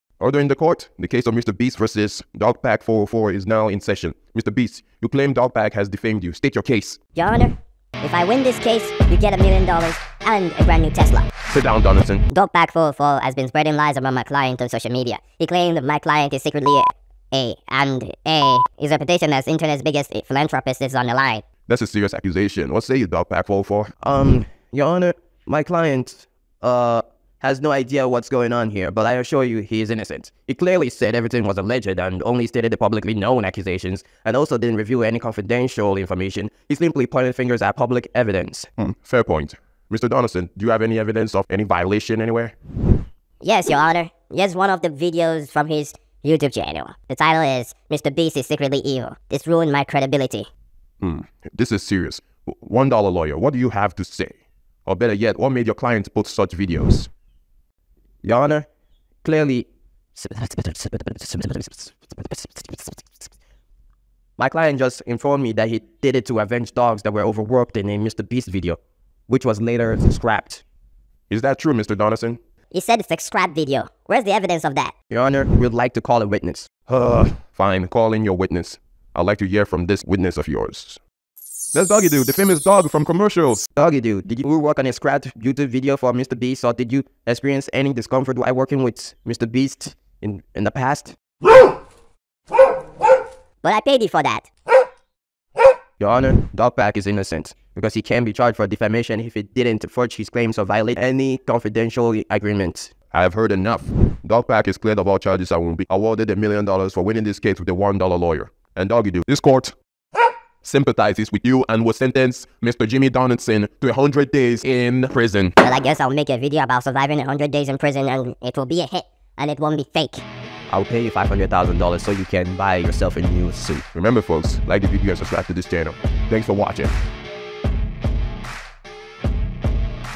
MrBeast $1,000,000 Lawyer VS Dogpack 404 $1 Lawyer
A parody video of MrBeast suing Dogpack 404 for defamation. ( MrBeast $1,000,000 Lawyer VS Dogpack 404 $1 Lawyer )